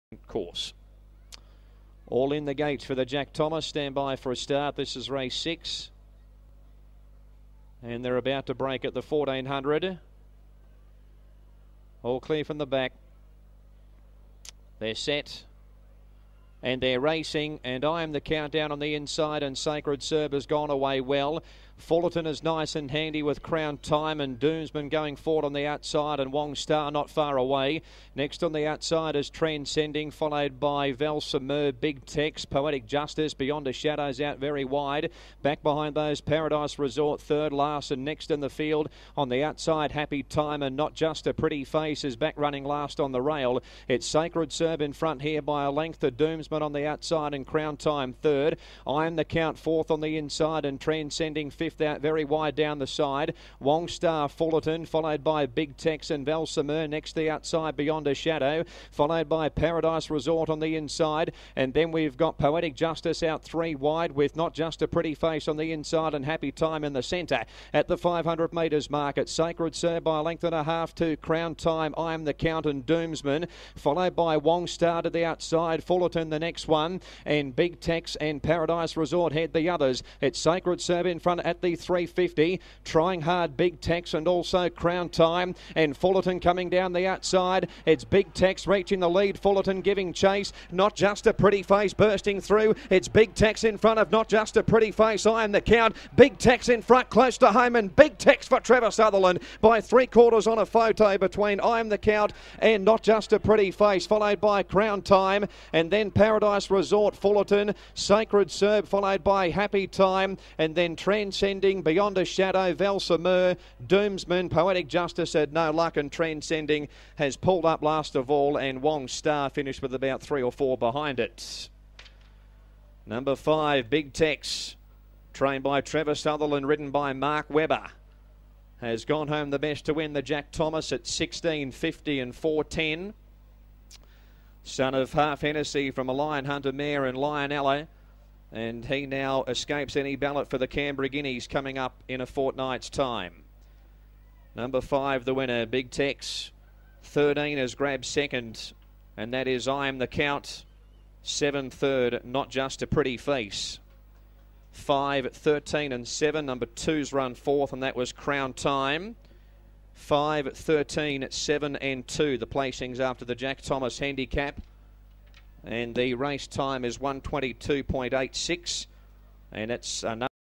R6 - Elders Commercial (BM63) - Big Tex
Age:3yo WT:54.0 Cls:Benchmark 63 API:2.60   $20,000
Rail:: Out 8m  1400m
Race Record: 1:22.87 Kriegspiel 03/11/2009 MDN(SW) SOT:G